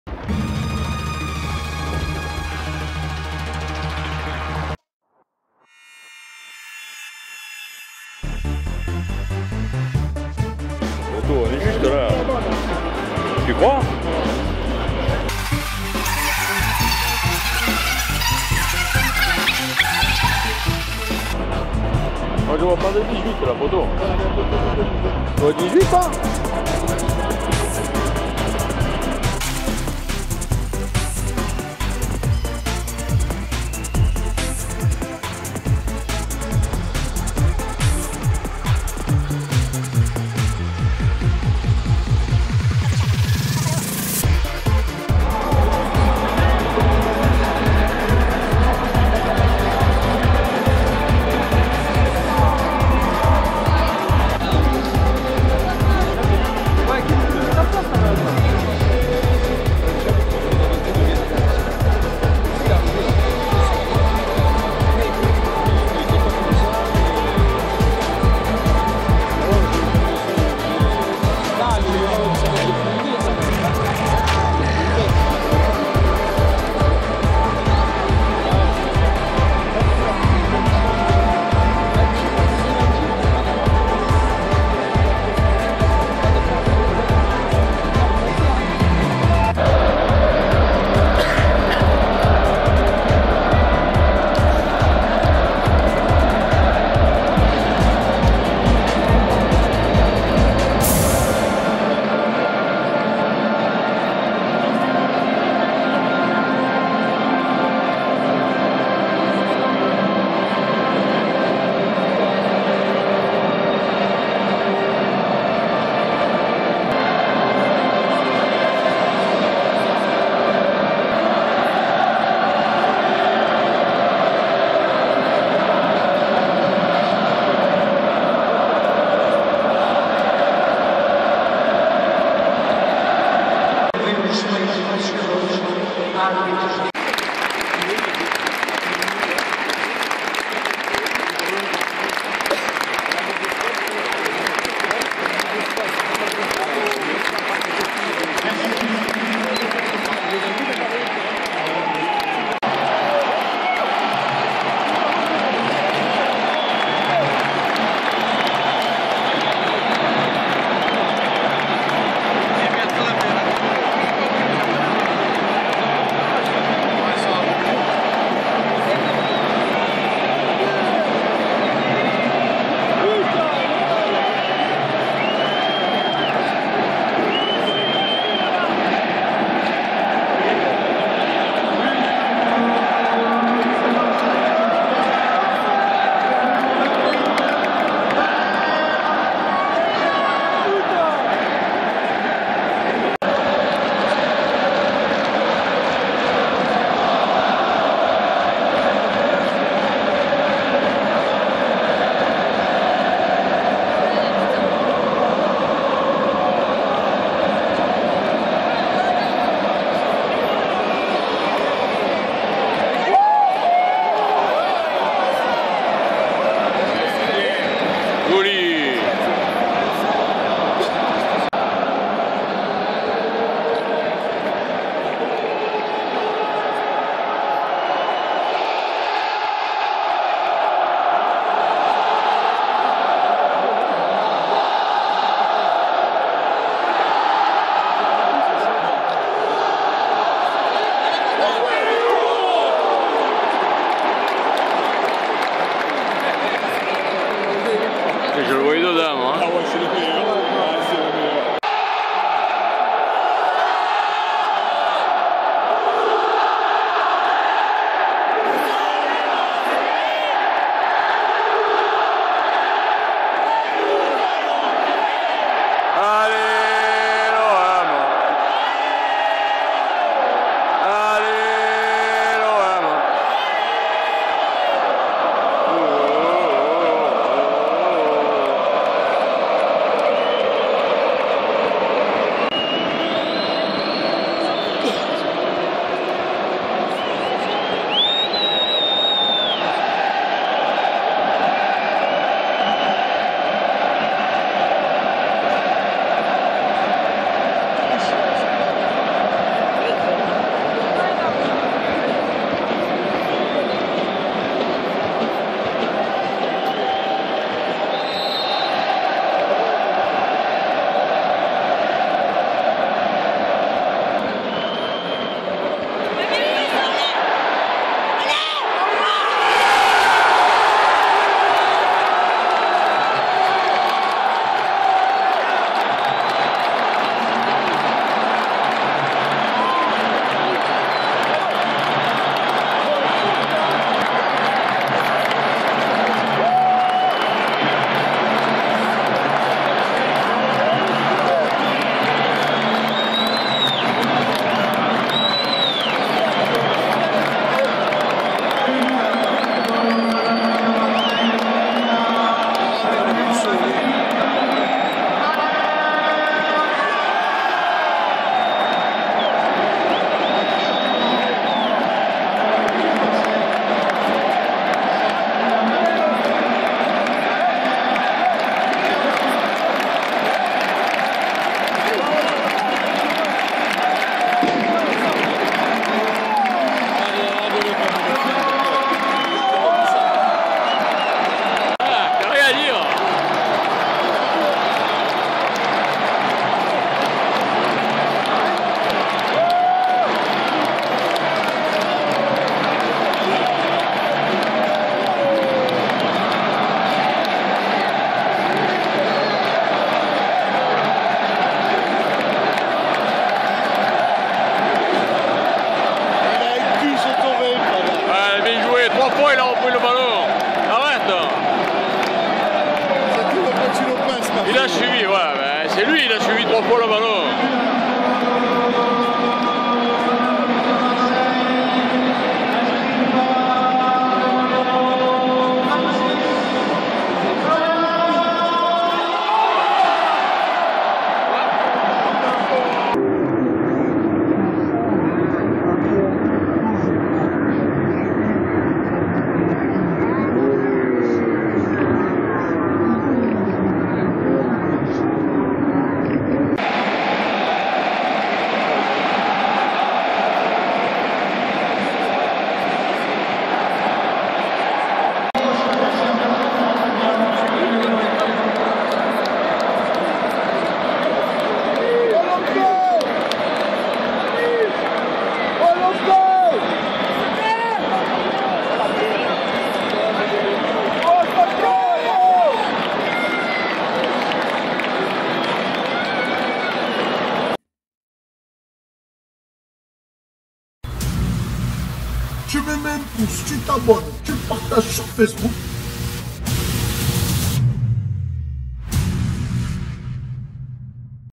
OM BRAGA 3 à 0 DOUBLé DE GERMAIN EN LIVE HD
L'OM s'impose au vélodrome face à BRAGA match allé des 16eme de finale de l'UEFA. on notera le doublé de Germain. et le 3eme but inscrit par FLOTOV (Florian Thauvin) 2 minutes après sa rentrée sur le terrain.